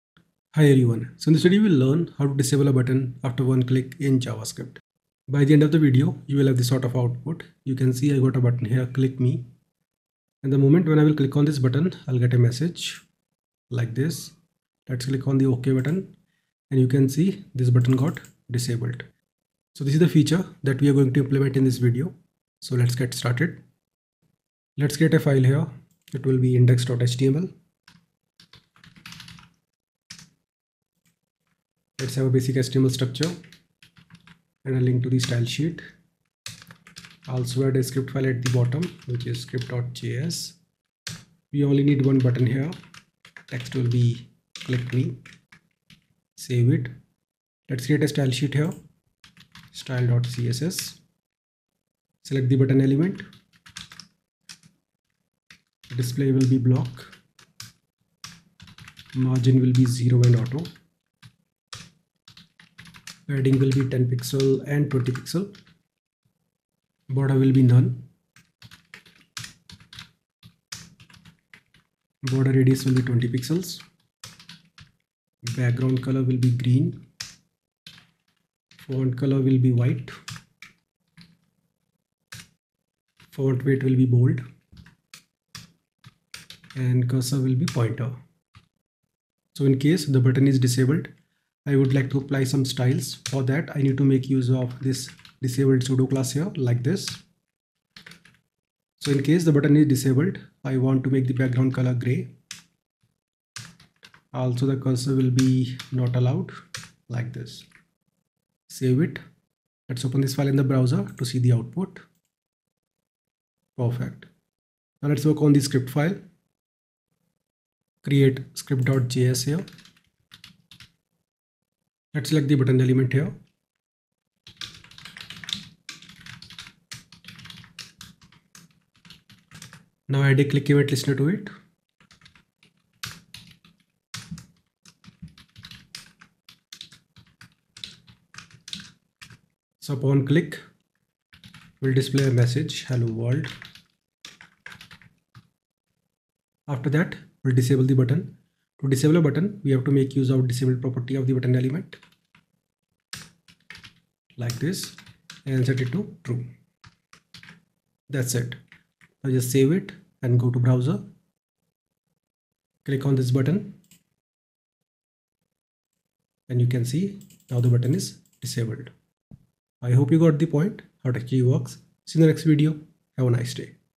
How to Disable Button After One Click in Javascript
In this video, you will learn how to disable button after one click in javascript. I hope you'll like the video and leave your feedback and suggestions for future web development related video tutorials.

0:00 Intro
0:24 HTML
0:47 CSS
1:57 Javascript
2:41 Final Output